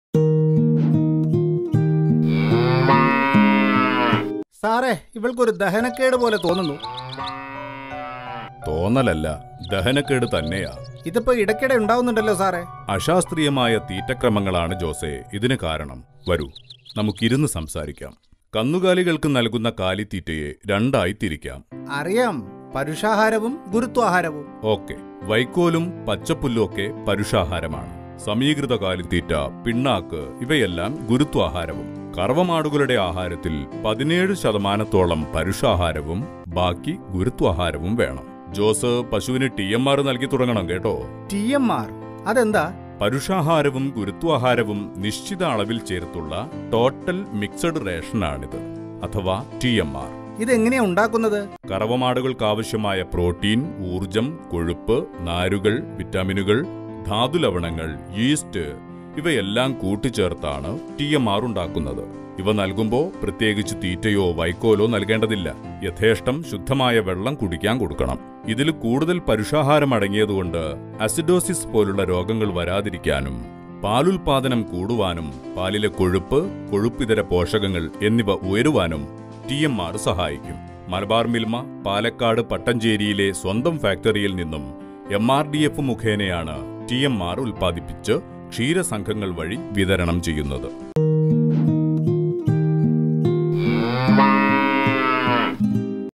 ഡോ.പശുപതി: ടി.എം.ആര്‍
കന്നുകാലികളുടെ തീറ്റക്രമമായ ടി.എം.ആറിനെ കുറിച്ചാണ് ഇന്ന് ഡോ.പശുപതി സംസാരിക്കുന്നത്.